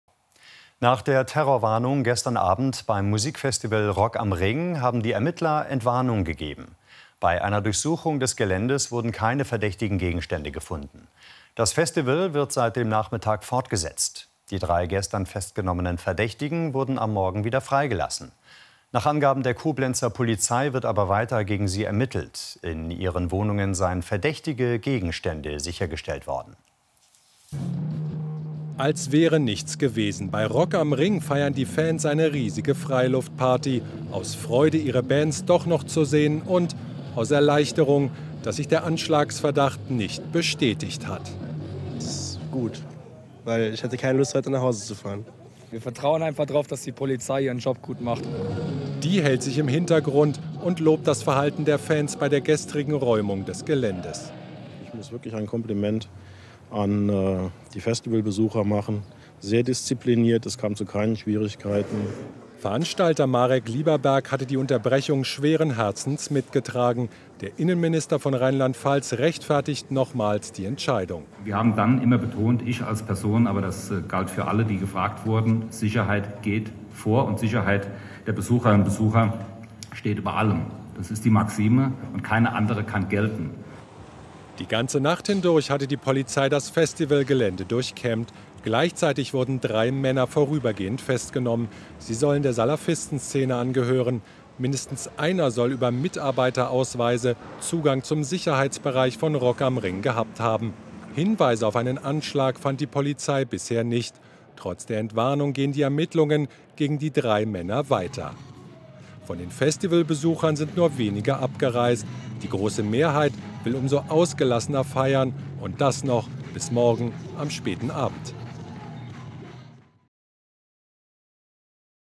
Rock am Ring 2017 - Terrorwarnung abgesagt
Freiluftpartie bei Rock am Ring, Ermittler haben nach Durchsuchung des Geländes keine verdächtigen Gegenstände gefunden. Gegen drei Salafisten wird ermittelt. Einer von ihnen hatte Zugang zum Sicherheitsbereich.

Nach Terrorwarnung: Entwarnung bei "Rock am Ring"-Festival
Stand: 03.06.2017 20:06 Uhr